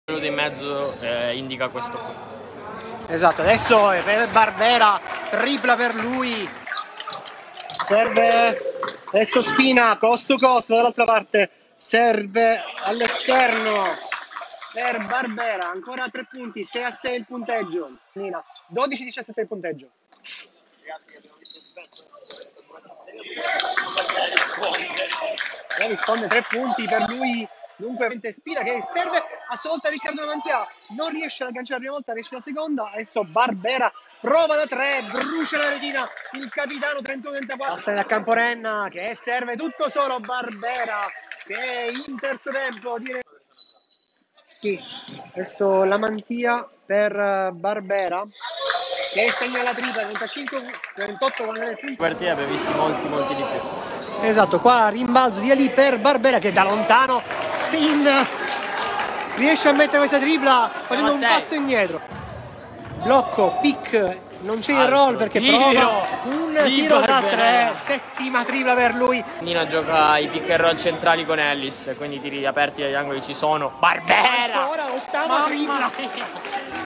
Gianluca Barbera: 27 punti, con 8/12 da tre, in Gravina-Orlandina Lab 82-83
Il capitano del Gravina, Gianluca Barbera, segna 27 punti con 8/12 da tre, nella partita tra Gravina e Orlandina Lab, 16. giornata di Serie C Silver, conclusa 82-83 il 23 maggio 2021.